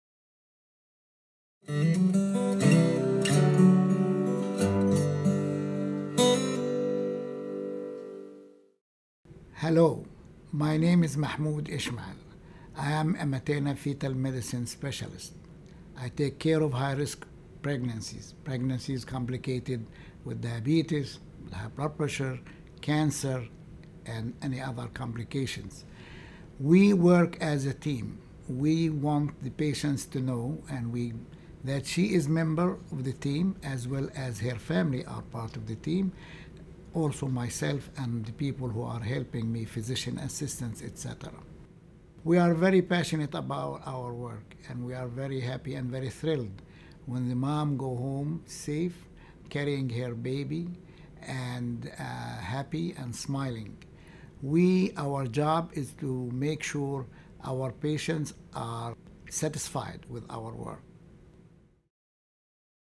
Mahmoud Ismail, MD - University of Chicago Medicine
Dr. Mahmoud Ismail is an expert in high-risk obstetrics, maternal-fetal medicine and infectious diseases in obstetrics. Actively involved in several areas of basic and clinical research, Dr. Ismail is studying the clinical outcomes of vaginal birth after cesarean section and breech vaginal birth versus cesarean section. He is also interested in bacterial infections in obstetrics, the immunology of pregnancy and pregnancy after bariatric surgery.